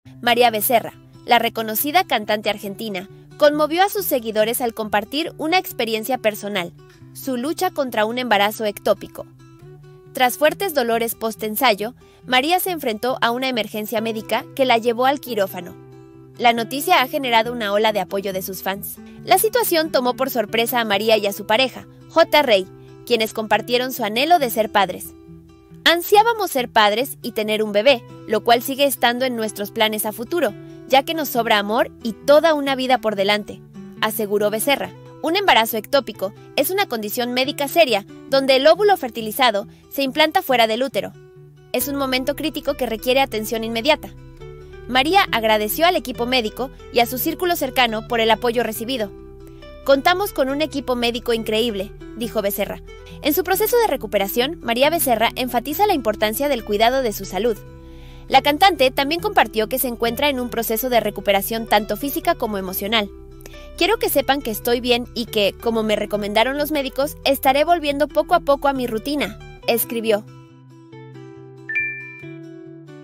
María Becerra y su lucha contra un embarazo ectópico
María Becerra revela su experiencia con un embarazo ectópico y su camino hacia la recuperación.
Encuentra los mejores Tips de belleza, viajes, comida, moda, diversión, etc. Consejos que te facilitarán la vida, A mí me lo dijo Lola ¿y a tí?